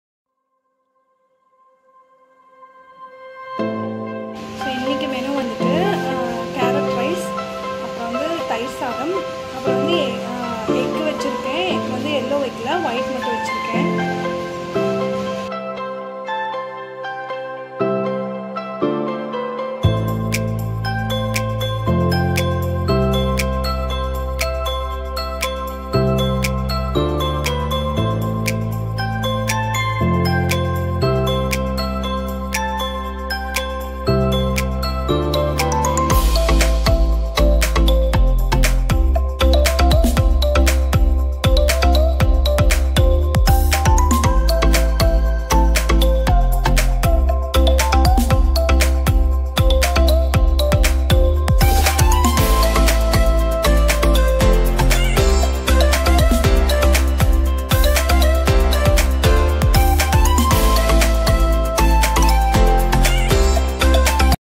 kids lunch box | lunch box recipes
In Lunch Box
🌼 Carrot rice
🌼 Curd rice
🌼 Egg

Friends, hope you all like this video ,so see you all in my next video .